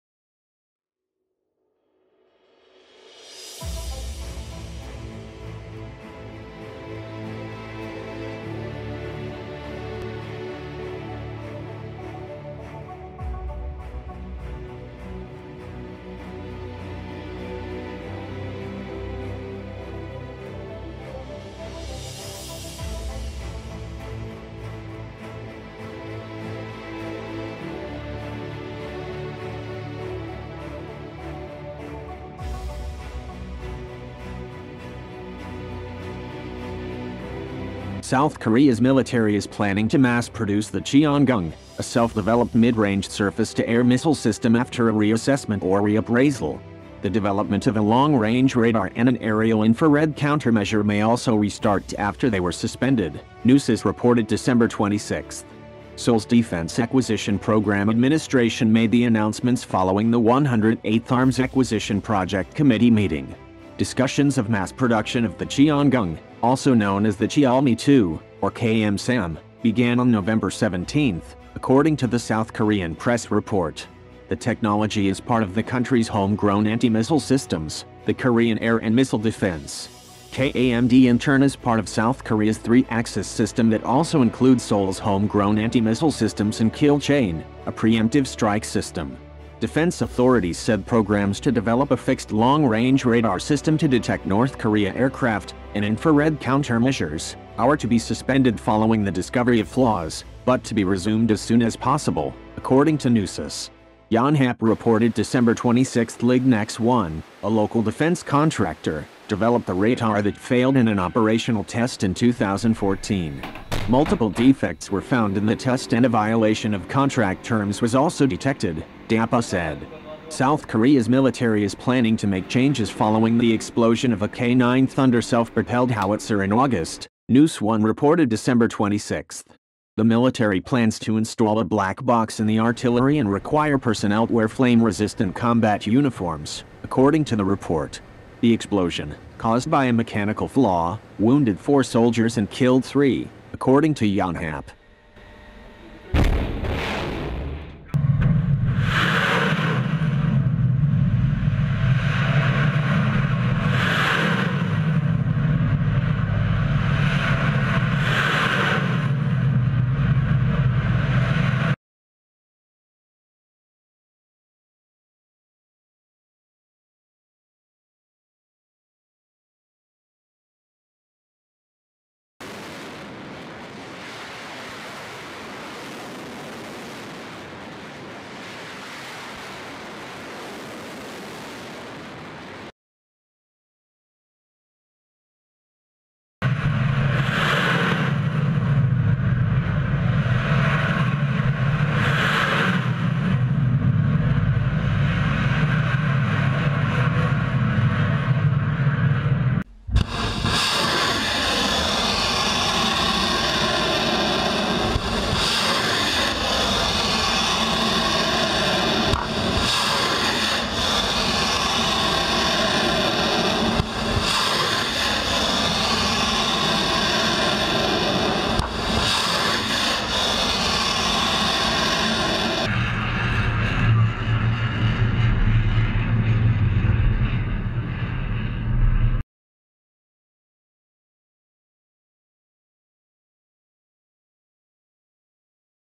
South Korea Ramps up Production of its own Anti Missile System
South Korea’s military is planning to mass produce the Cheongung, a self-developed midrange surface-to-air missile system after a reassessment or reappraisal.

The development of a long-range radar and an aerial infrared countermeasure may also restart after they were suspended, News reported Dec. 26.

Seoul’s Defense Acquisition Program Administration made the announcements following the 108th arms acquisition project committee meeting.